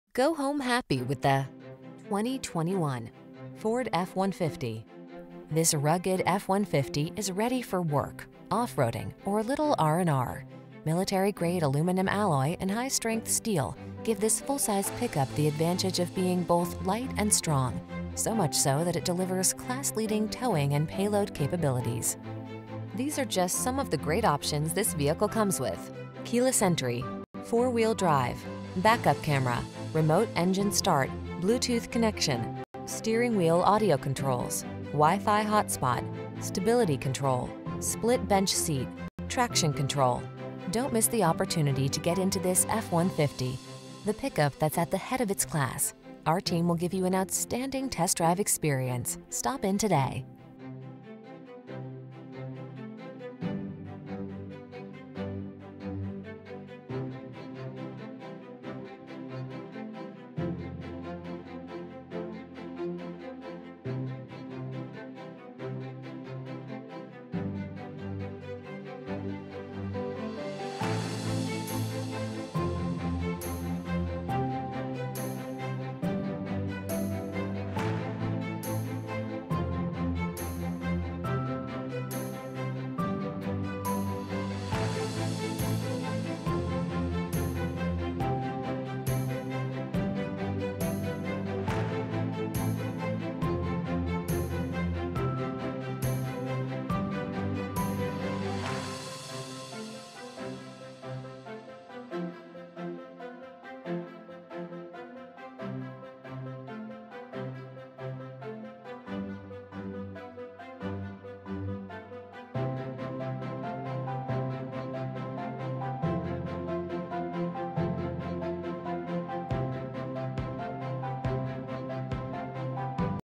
2021 Ford F-150 Columbus, Reynoldsburg, Westerville, Dayton, Springfield, OH 25668
Velocity Blue Metallic New 2021 Ford F-150 available in Columbus, Ohio at Dick Masheter Ford. Servicing the Reynoldsburg, Westerville, Dayton, Springfield, OH area.
Used:
New:
2021 Ford F-150 XL - Stock#: 25668 - VIN#: 1FTEW1EP9MKD20900